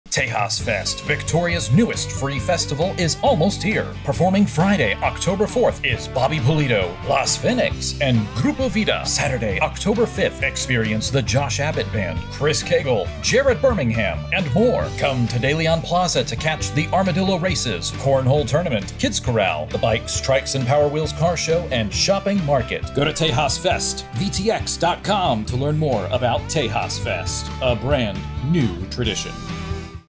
Tejas Fest 2024 Commercial Spot
Come on out to Victoria's newest annual tradition, Tejas Fest, happening this October 4-5, in De Leon Plaza! Featuring a huge lineup of Country and Tejano music legends like the Josh Abbott Band, Chris Cagle, and Bobby Pulido, dozens of food and market vendors, a kids' corral, car show, cornhole tournament, tamale-eating contest, armadillo racing, a petting zoo, Lucha Libre, and so much more! Best of all, the event is completely FREE!